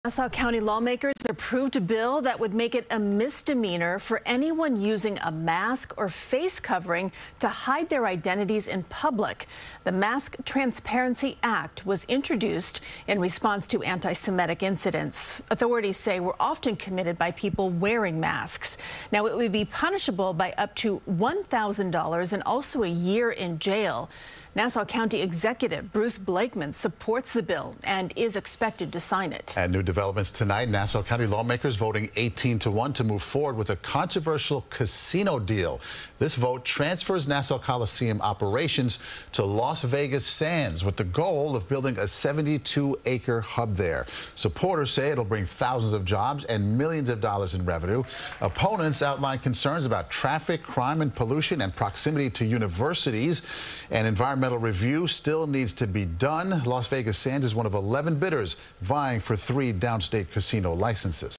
Long Island lawmakers pass bill banning face masks
The Mask Transparency Act was introduced in response to antisemitic incidents authorities said were often committed by people wearing masks.